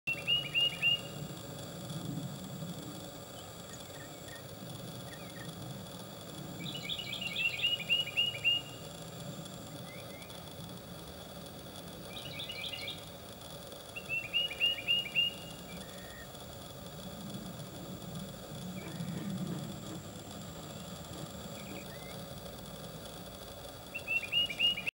Sandhill Crane Baby
Salt Springs, Fl. Sandhill Crane young chick flapping wings!